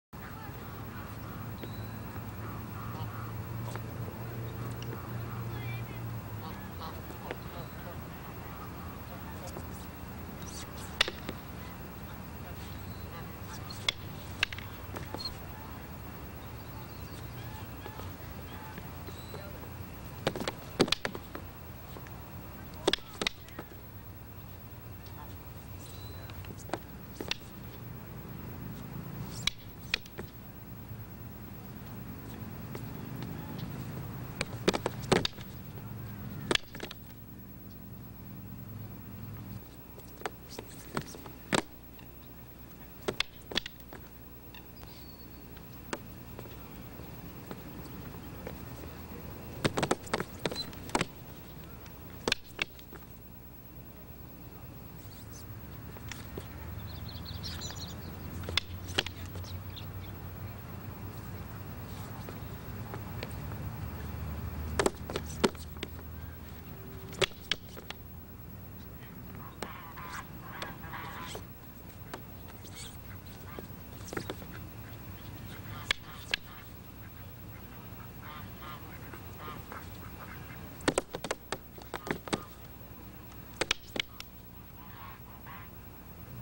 Pure Art of Serrada ng Escrima: Advanced Weapons Training (A
One of the most unique training methods Father of Escrima, Angel Cabales, taught me was how to develop a fighter’s depth and breadth perception. As a Master Trainer, there are several training methods I utilize to enhance the fighter’s fighting skills and abilities. Deceptions, fakes, and devious attacking movements completing with the Triple strike to the head provides the essential dimension to heighten the fighters mindfulness and controlled reactiveness to the real and unreal attacks.